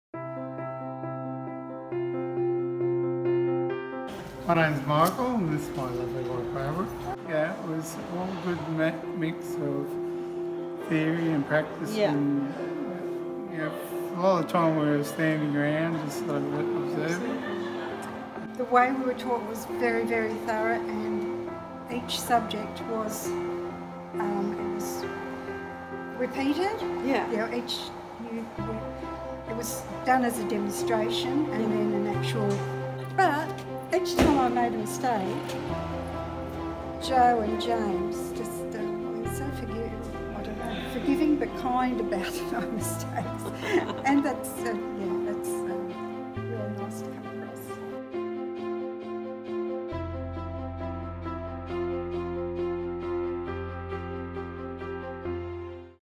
Tiny House Building Course Review - Michael & Barbara
The Bower's Tiny House Building Course is a 6 day hands on building course with reclaimed material. In 2020, 15 participants took part and learned some valuable lessons. Here's what one of the participants Michael & Barbara had to say!